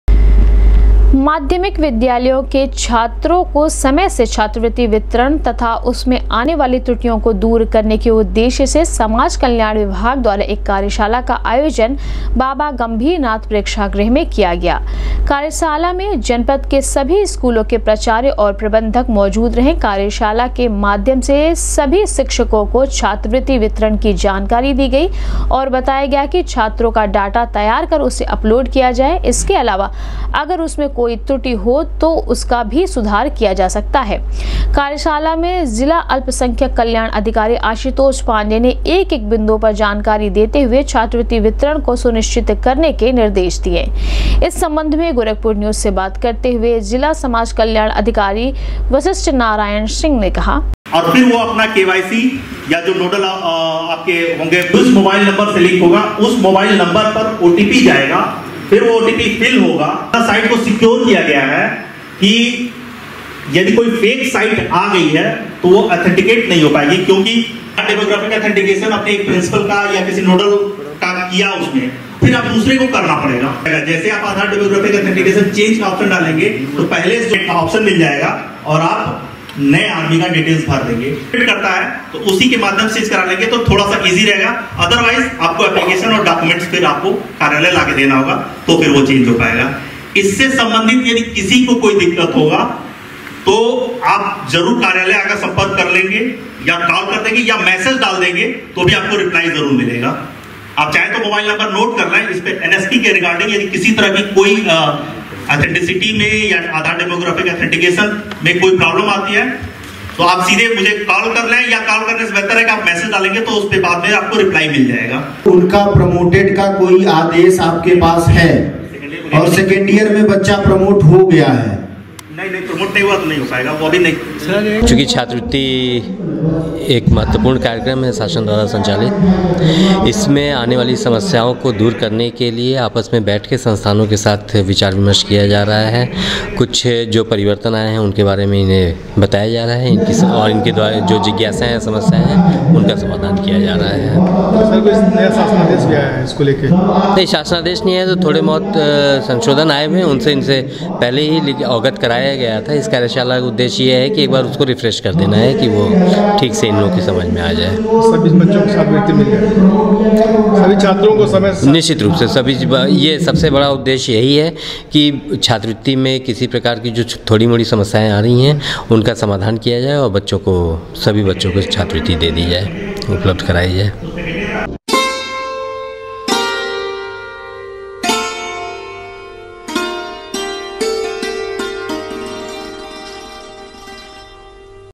समाज कल्याण विभाग के तत्वावधान में आयोजित हुआ छात्र वृत्ति वितरण से सम्बन्धित कार्यशाला, दी गयी
समाज कल्याण विभाग के तत्वावधान में आयोजित हुआ छात्र वृत्ति वितरण से सम्बन्धित कार्यशाला, दी गयी छात्र वृति वितरण सम्बन्धित जानकारी